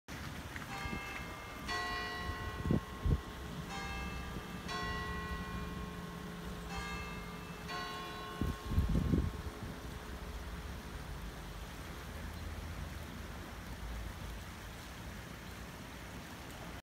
Near Abbey of Lonlay l'Abbaye
Old wooden bridge across the small river at Lonlay l'Abbaye, Normandy, France